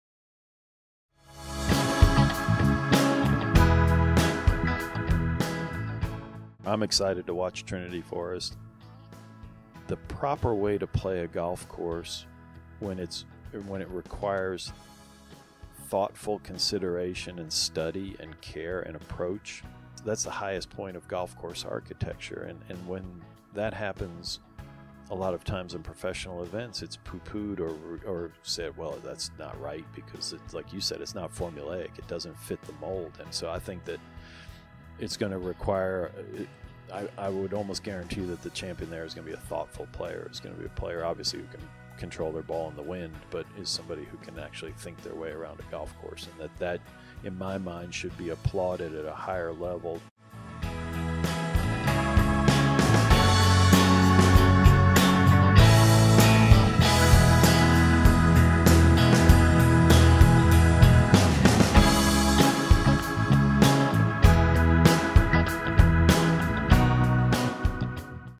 Trinity Forest Golf Club
Gil Hanse describes host of the PGA Tour's AT&T Byron Nelson, Trinity Forest Golf Club.

Sign up for The Fried Egg Newsletter to keep up to date with all things golf and golf architecture here: